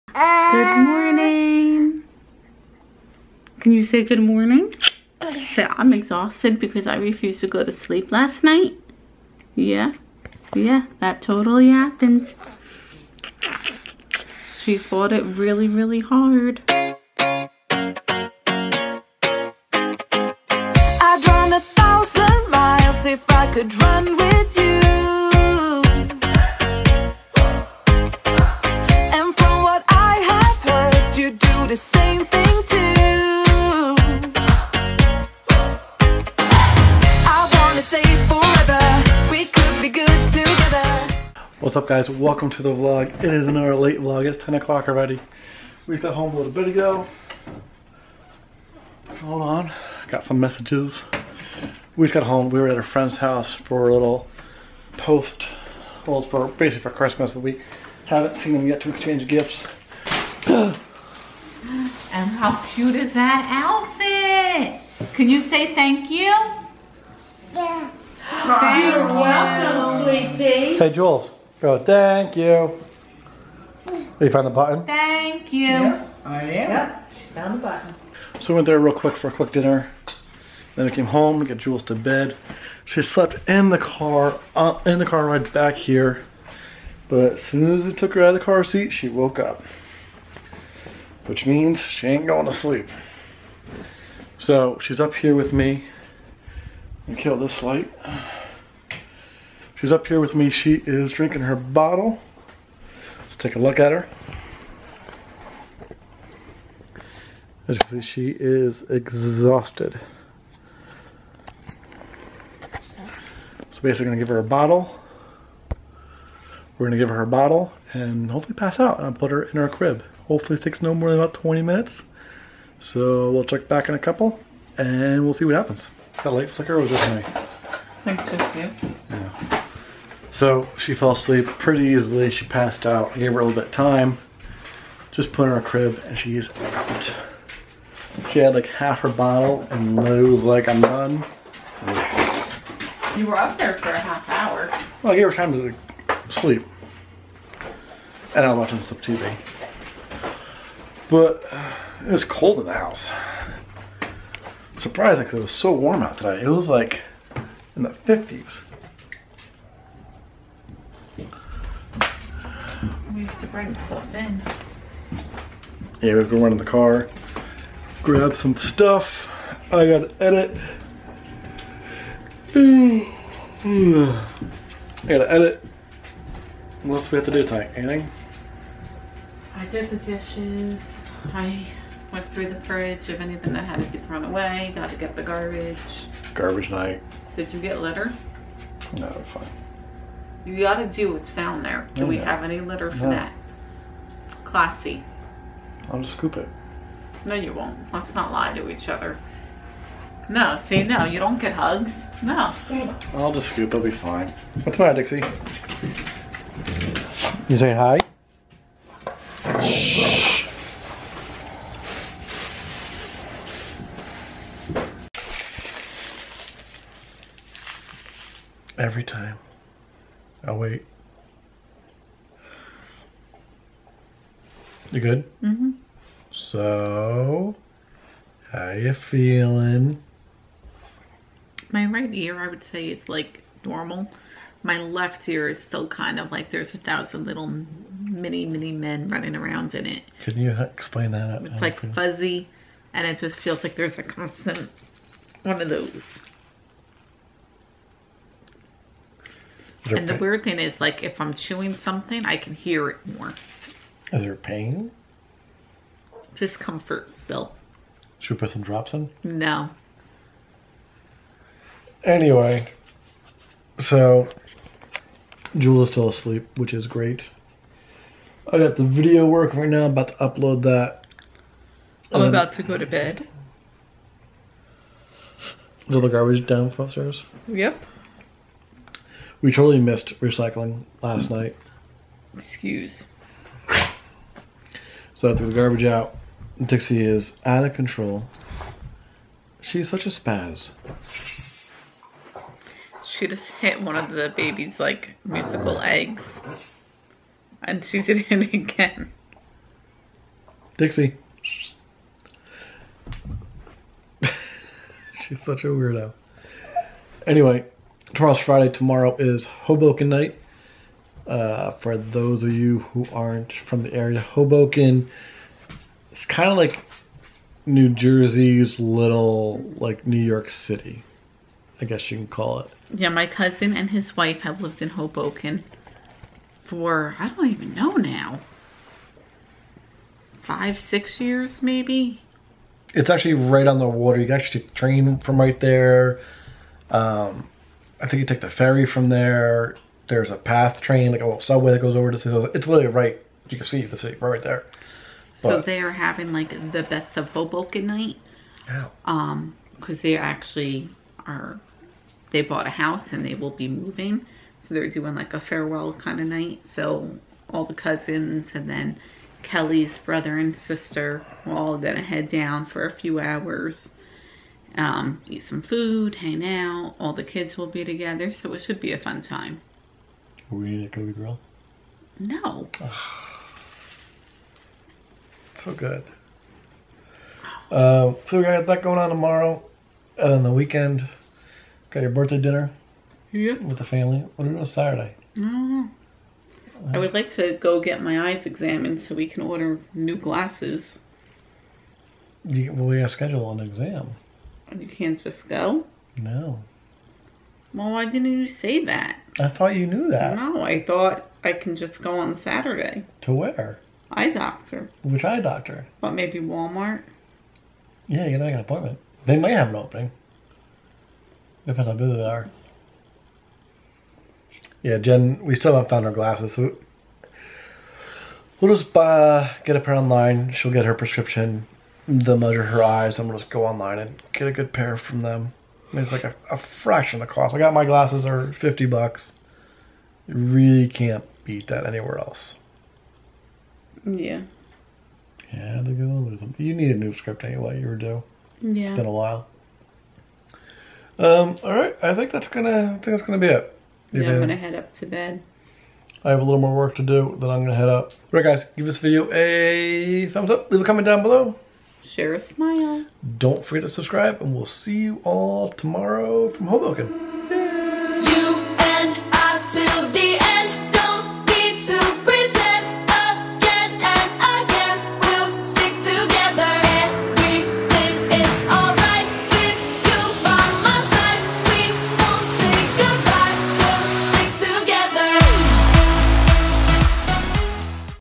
POST SURGERY UPDATE!
Jenn claims it feels like a thousand little people jumping around in her ear. whatever that means!